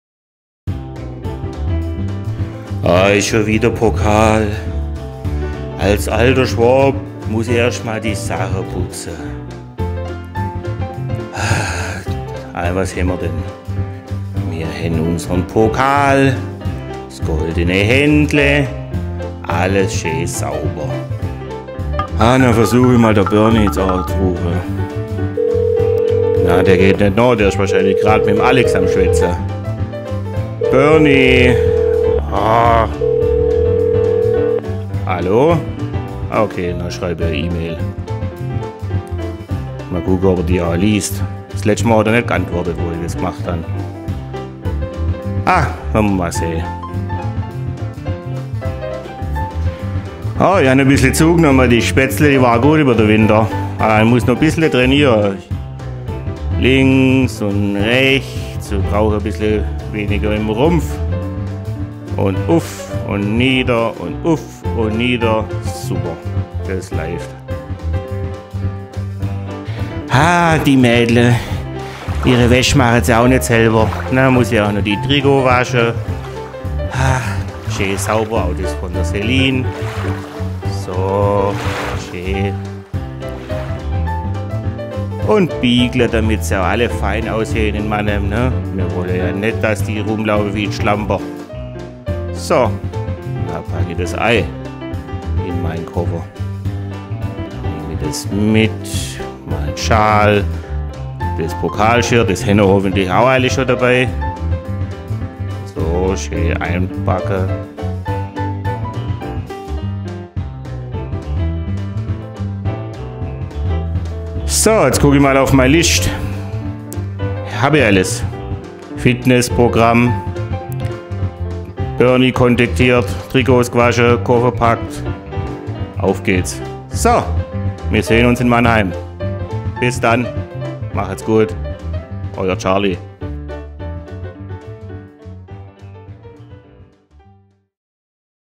Teaser DVV-Pokalfinale 2020
Charly bereitet sich auf die Reise nach Mannheim zum DVV-Pokalfinale vor und nimmt uns mit. Seid ihr auch bereit?